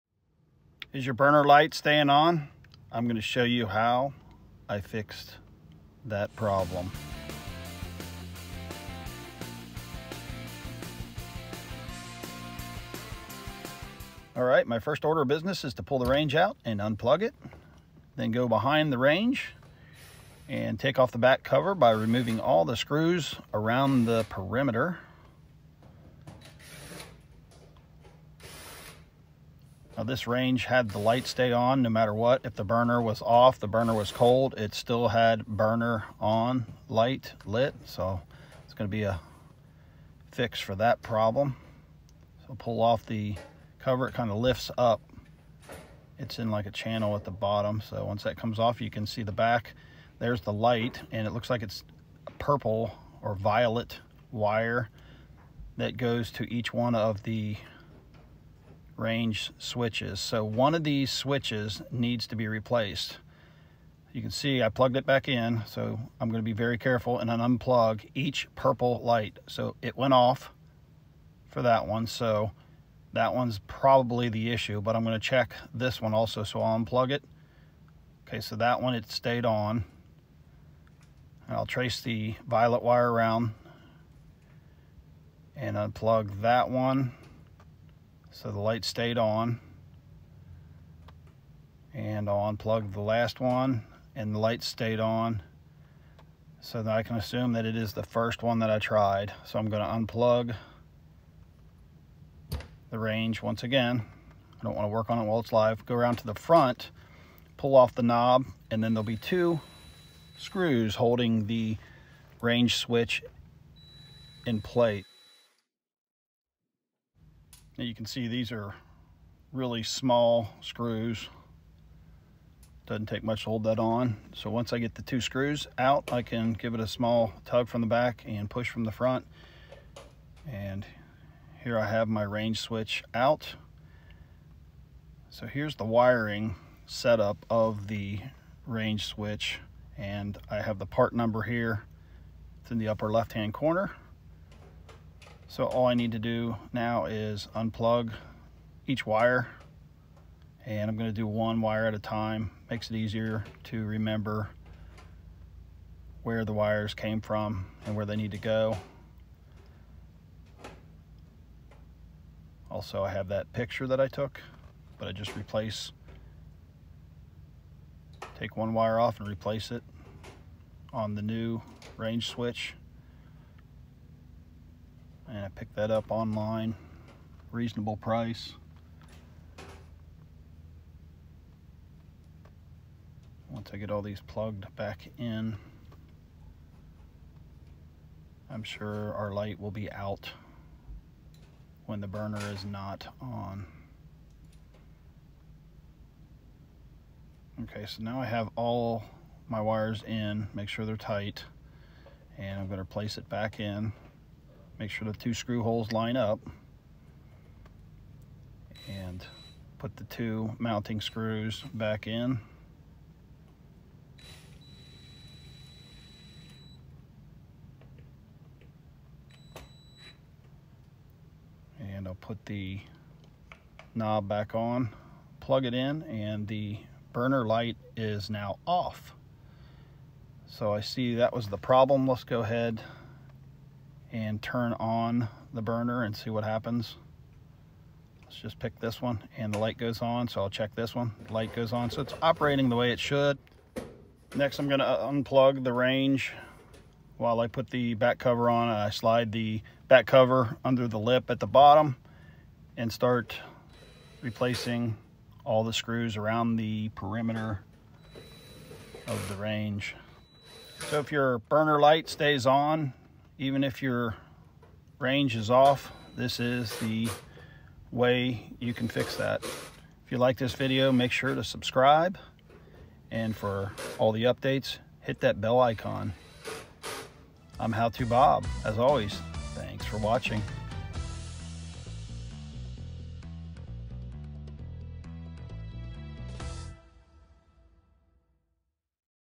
Stove Burner light stays on, FIX
In this video I will show how I troubleshoot and fix a burner light that stays on. This repair is being done on a Hotpoint Range.